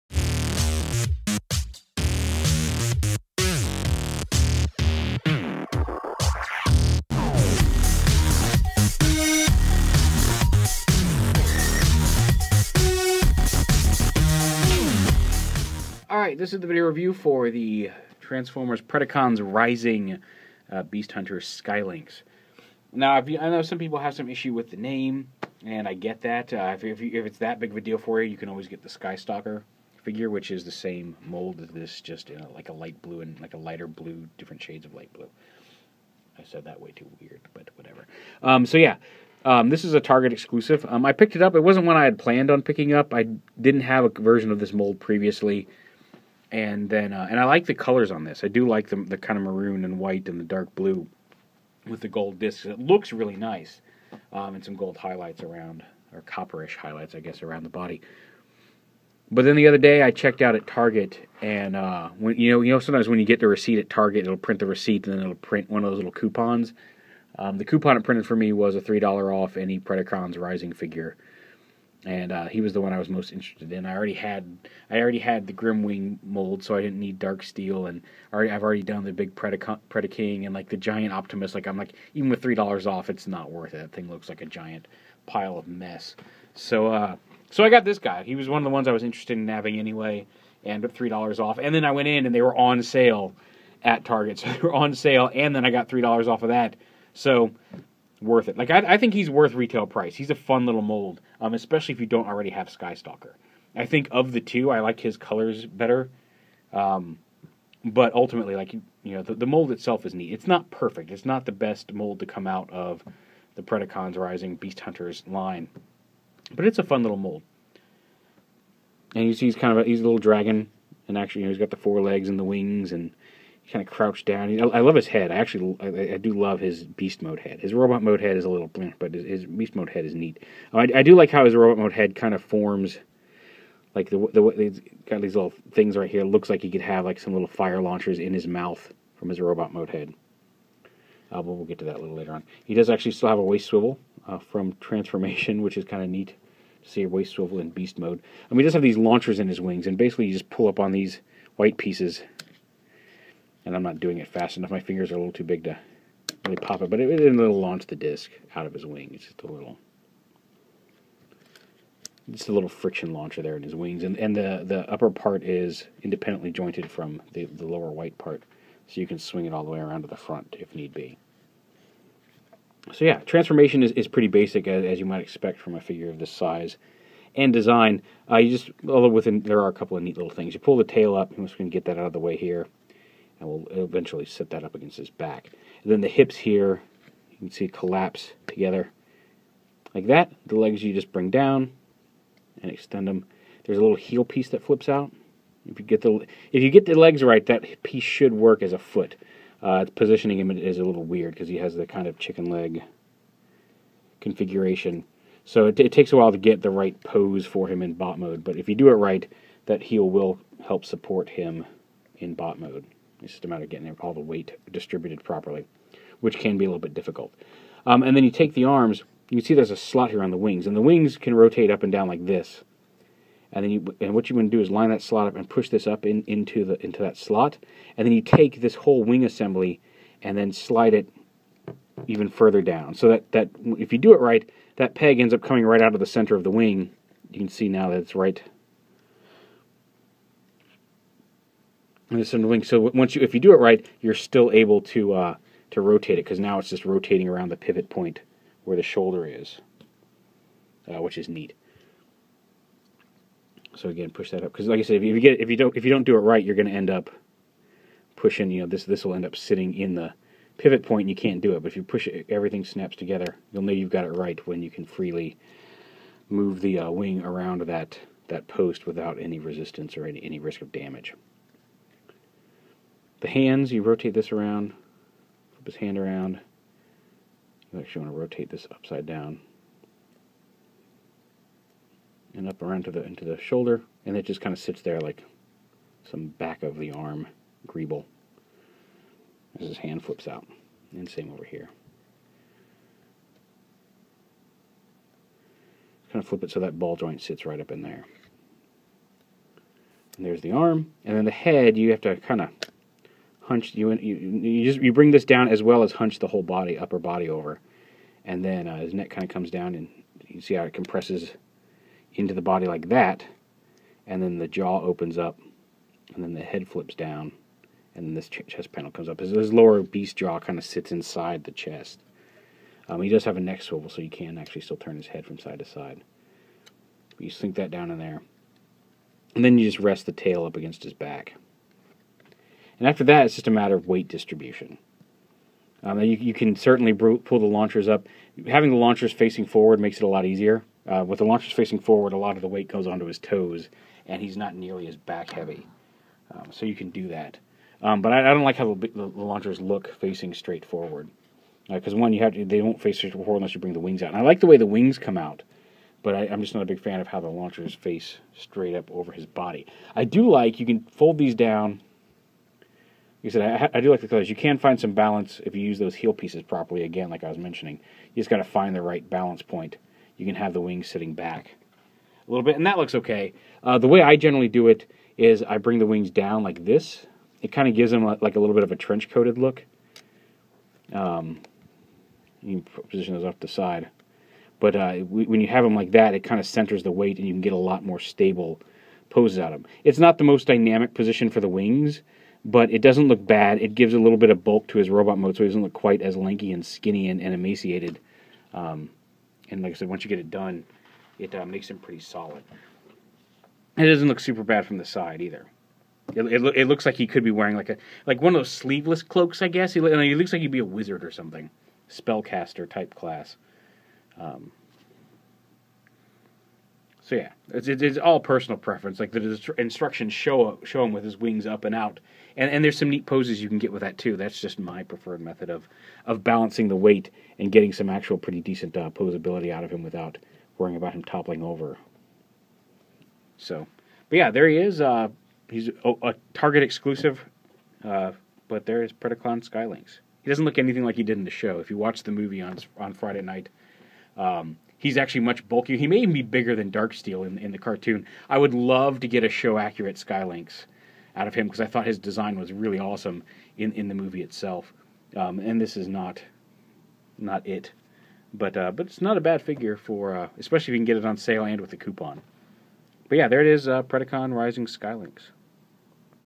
Transformers Prime - Predacons Rising SKY LYNX
A look at the Target exclusive repaint of Skystalker!